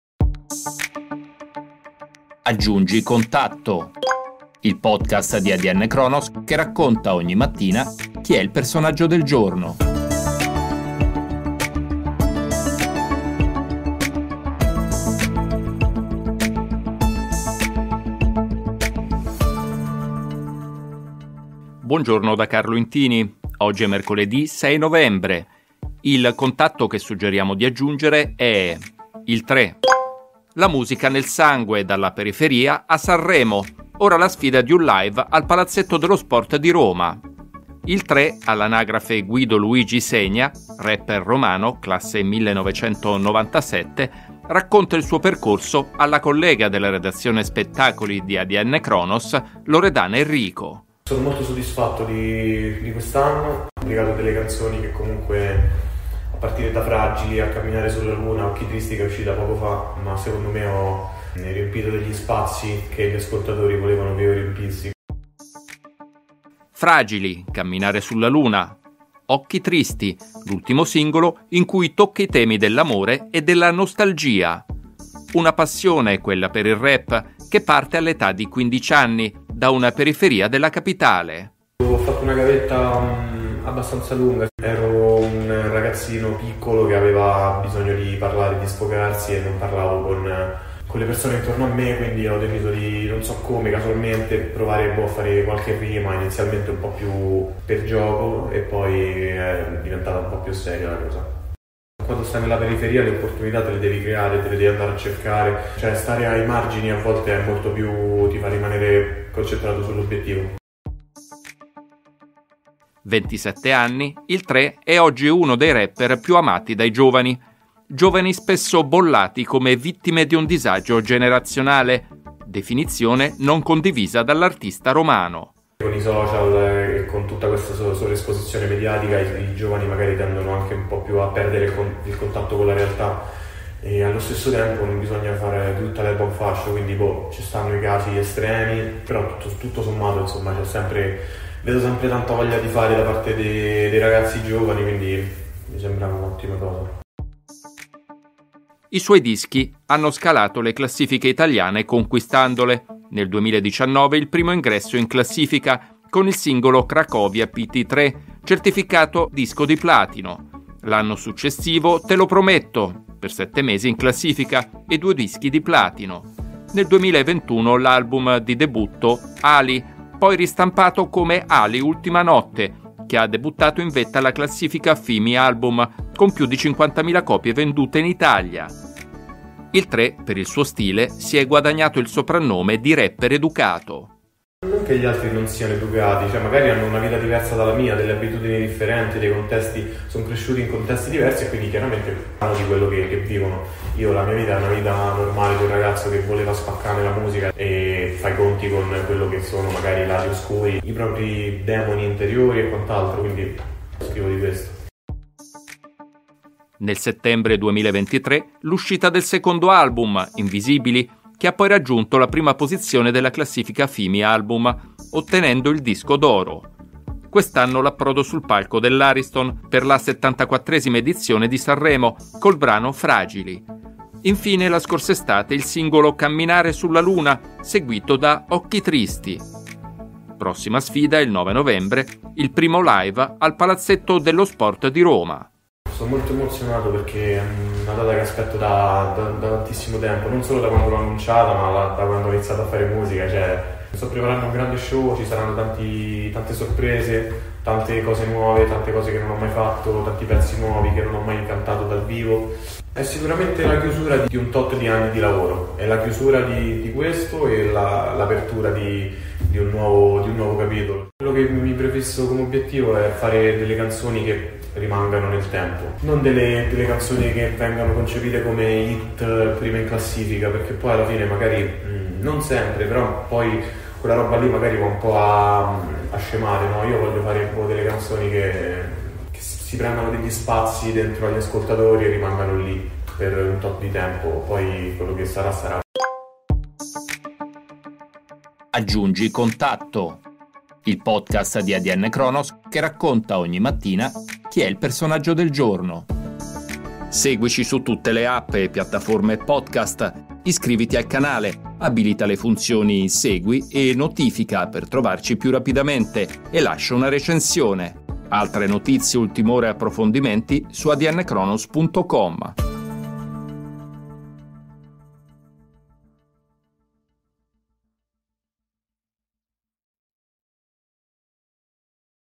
Il Tre, dalla periferia al sogno, pronto il live al Palazzetto dello Sport - Aggiungi contatto -
La musica nel sangue, dalla periferia della Capitale a Sanremo, ora la sfida di un live al Palazzetto dello Sport di Roma. Il Tre, all’anagrafe Guido Luigi Senia, rapper romano classe 1997, racconta all'Adnkronos il suo percorso artistico ed i suoi piani futuri.

Musiche su licenza Machiavelli Music.
AdKey:zP-94qNWQqj3XM